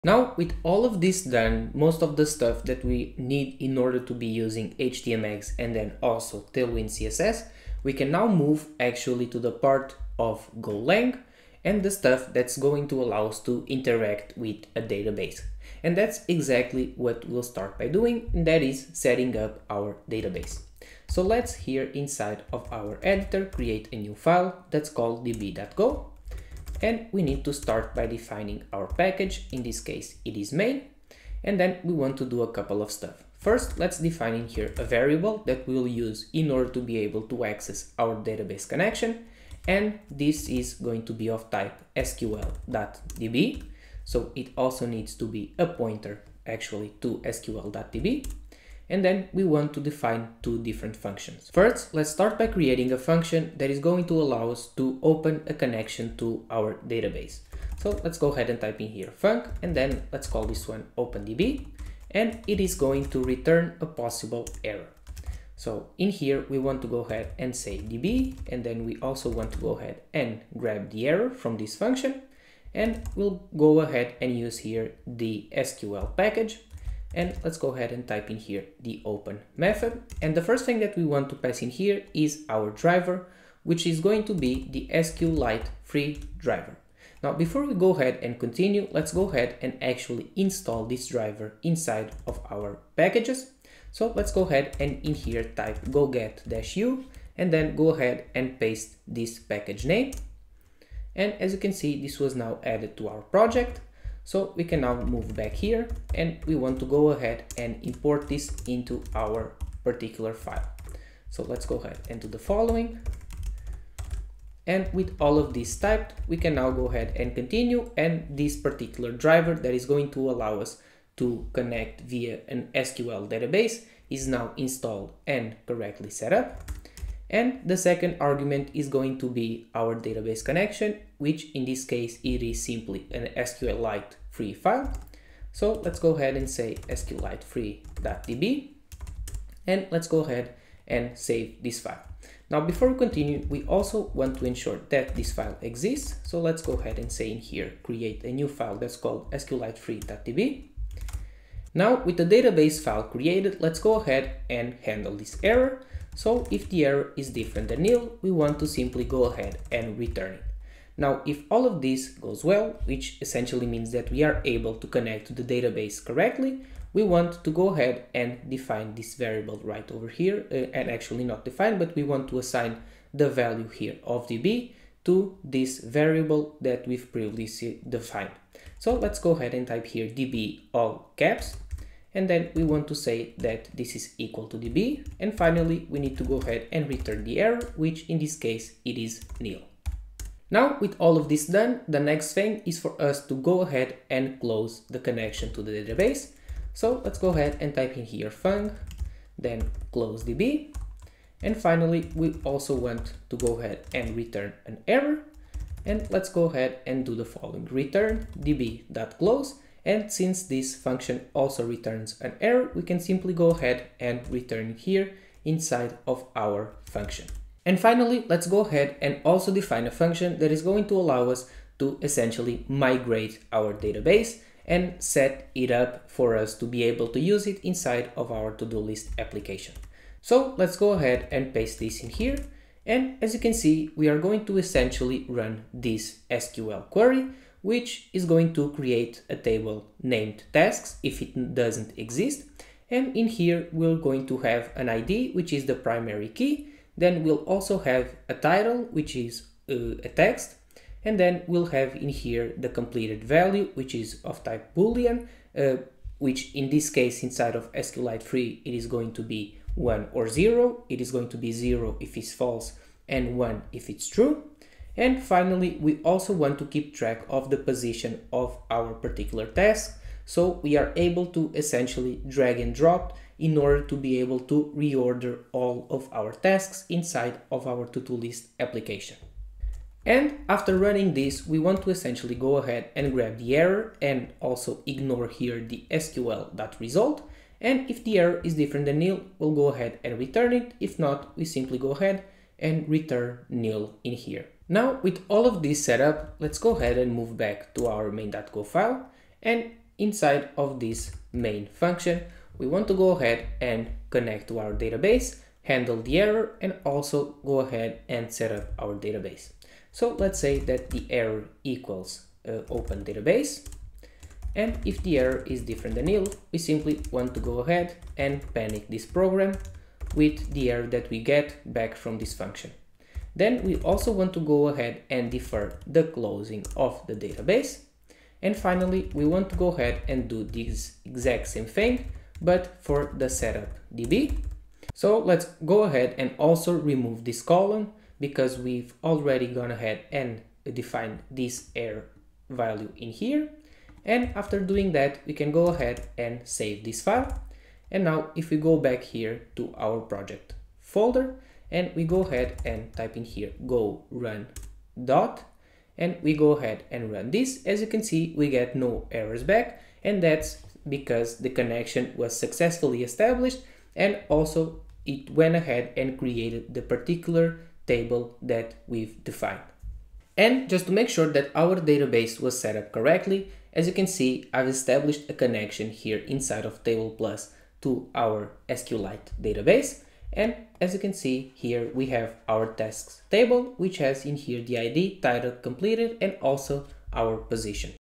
HTMX Go Todo List Application (#3 - SQLite Database)
In this video, of the HTMX Go Todo List Application series, you'll learn how to conncect to SQLite and migrate the database.

00:00 - Define DB variable
00:32 - Define function to connect to DB
03:11 - Define function to disconnect from DB
03:37 - Define function to setup/migrate DB
04:54 - Run previous functions inside main.go